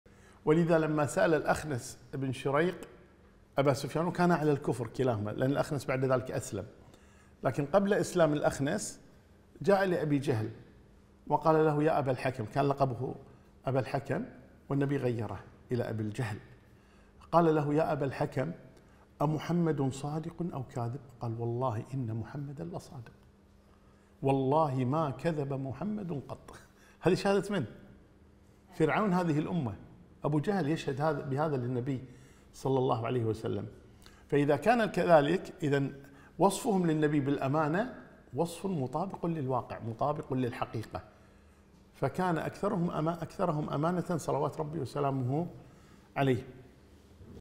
9 - شهادة فرعون هذه الأمة على صدق النبي محمد ﷺ - عثمان الخميس
هذه الفائدة مستخرجة من محاضرات الشمائل المحمدية

حسابات الشيخ عثمان الخميس في مواقع التواصل
1_فيس بوك